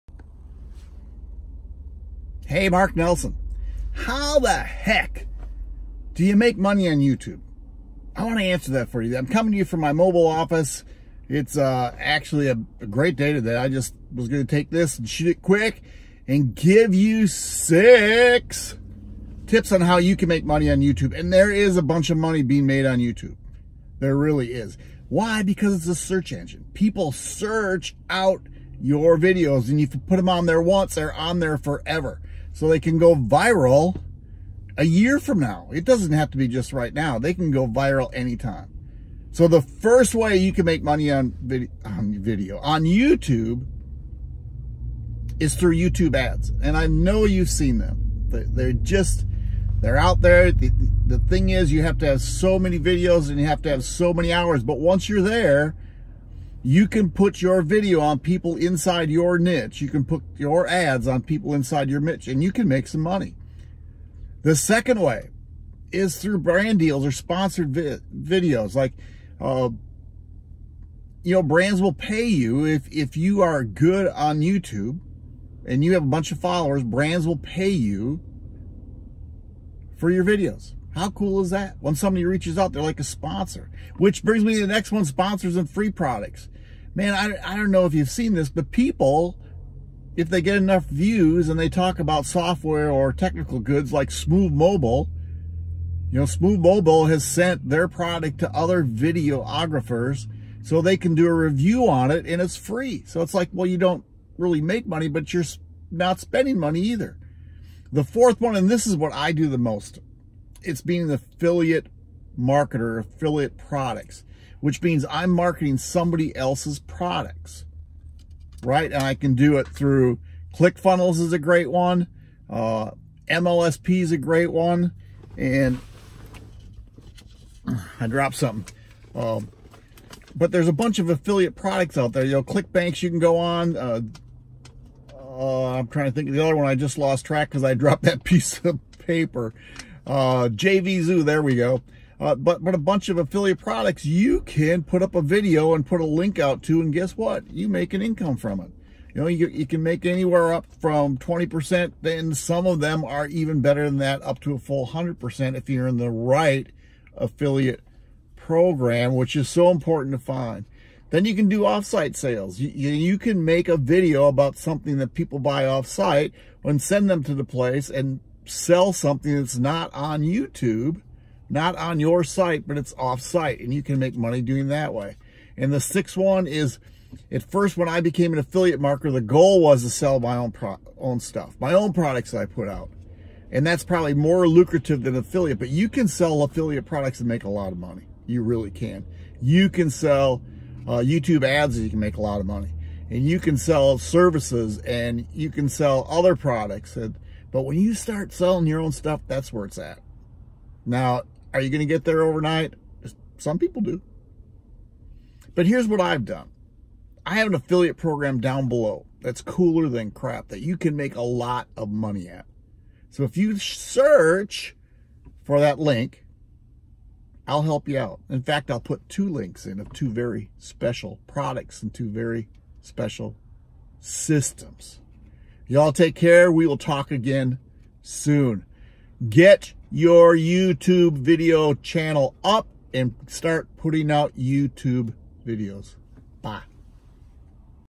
How To Make Money On YouTube
I get asked this all the time how to make money on YouTube so I shot a quick video to give you 6 tips on how you can make money on Youtube

Looking for Freedom?

Here is a free web class that shows you step by step